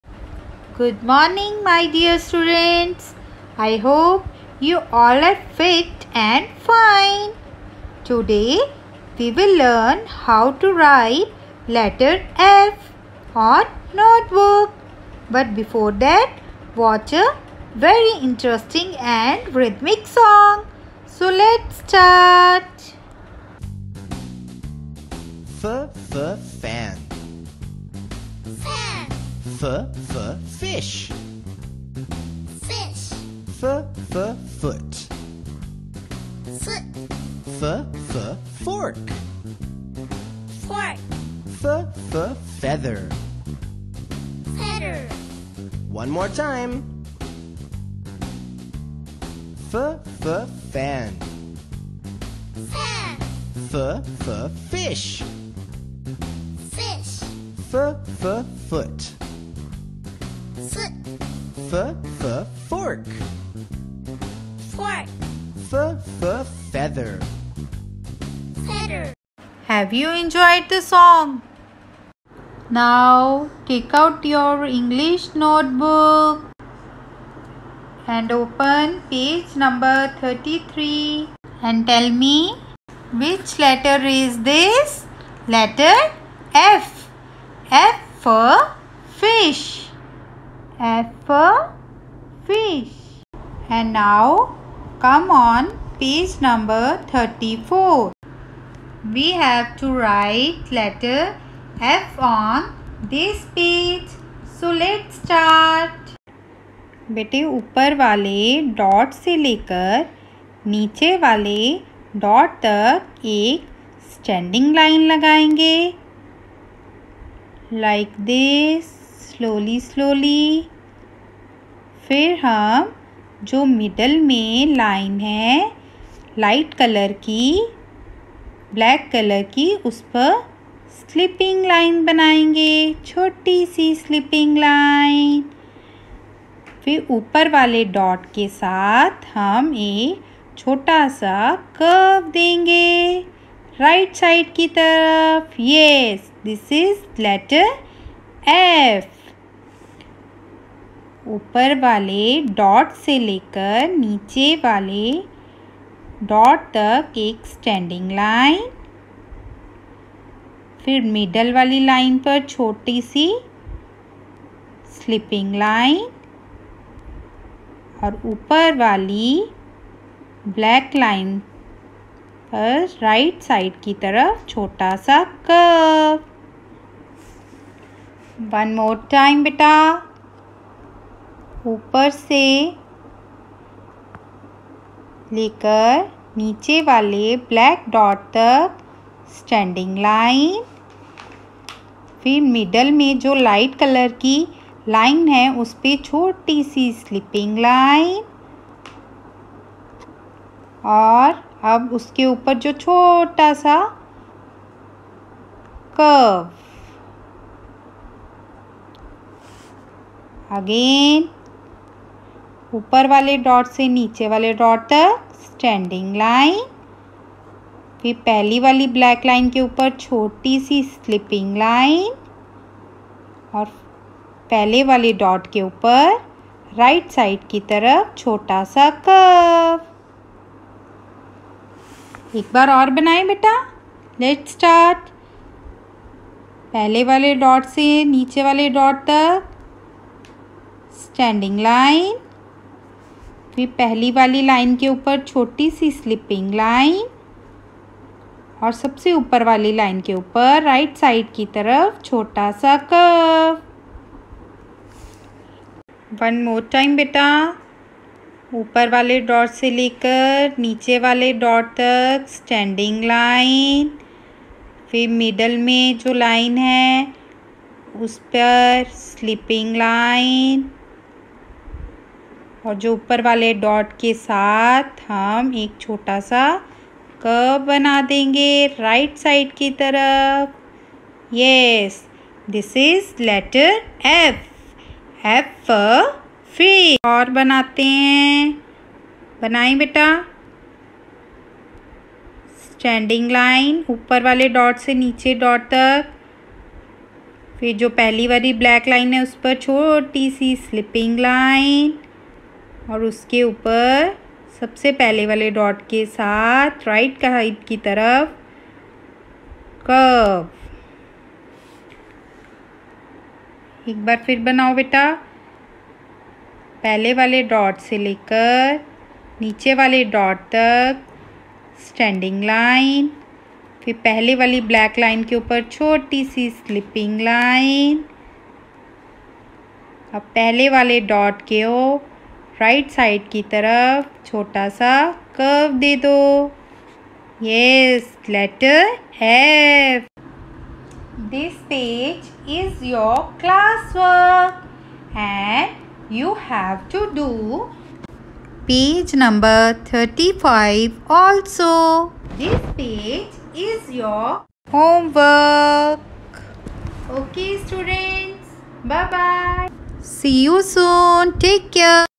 Letter ‘f’
Learn how to write letter ‘f’ on notebook